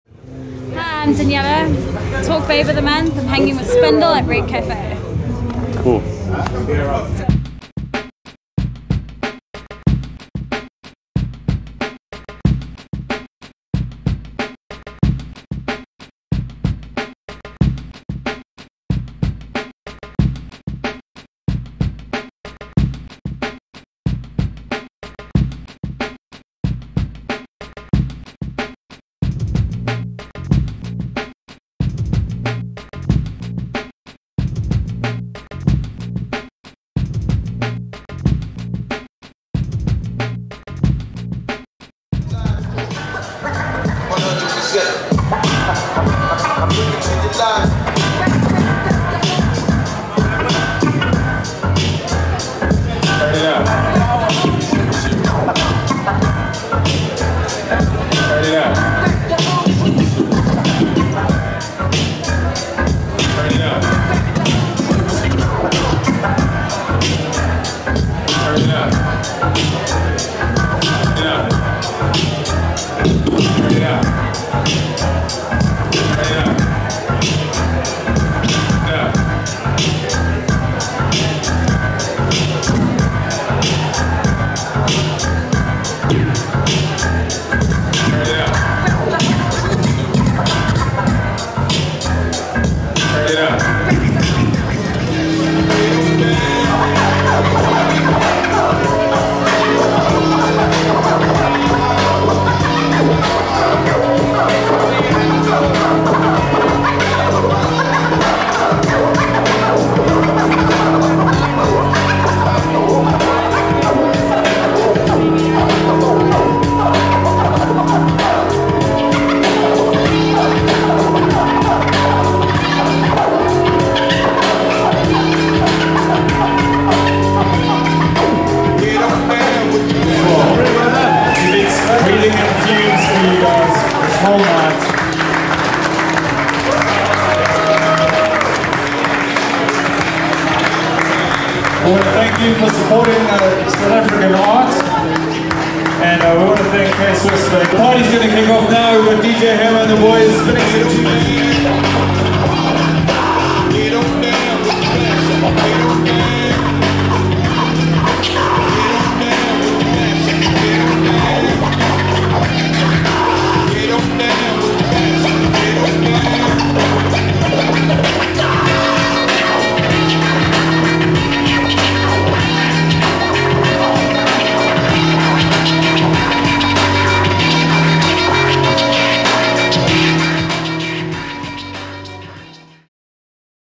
Spindle Mag Feat. K Spray Volume 6 Part 5/13
K - Swiss held a shoe design competition in December 2006 in Cape Town. The Event was called K-Spray. DJ Hamma was there to play the beats.